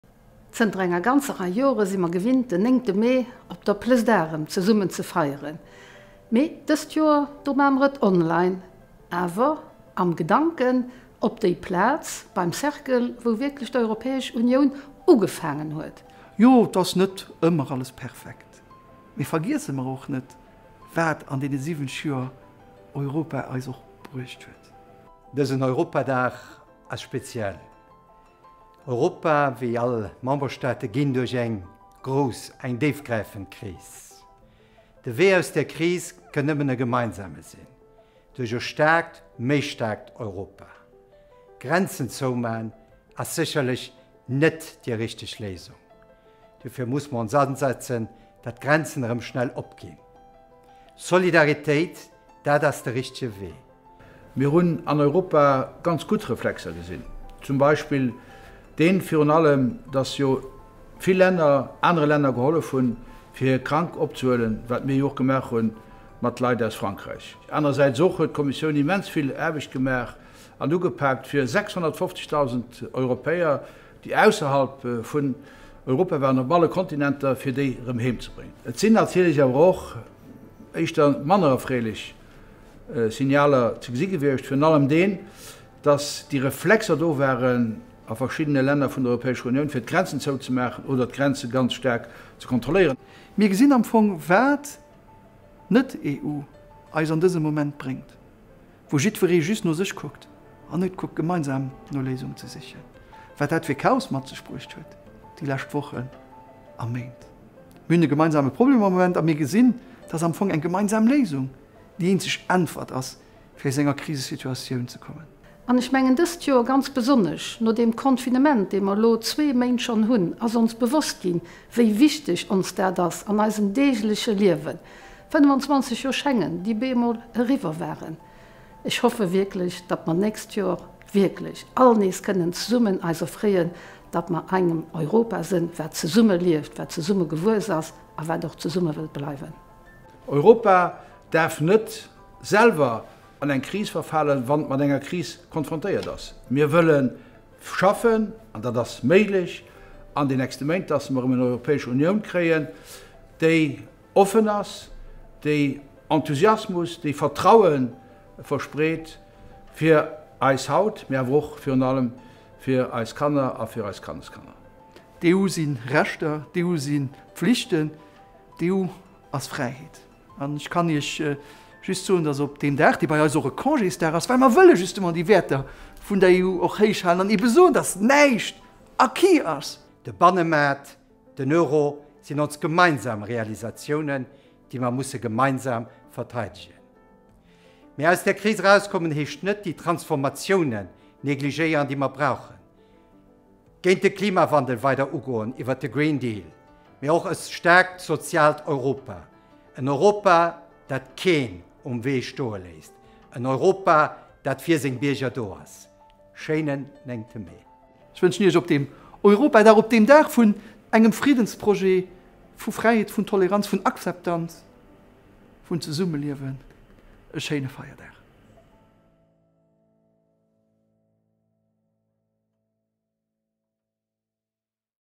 Journée de l'Europe
Aujourd'hui, c'est la journée de l'Europe. L'occasion pour le Luxembourg de rappeler son attachement à l'idée européenne en ces temps de crise.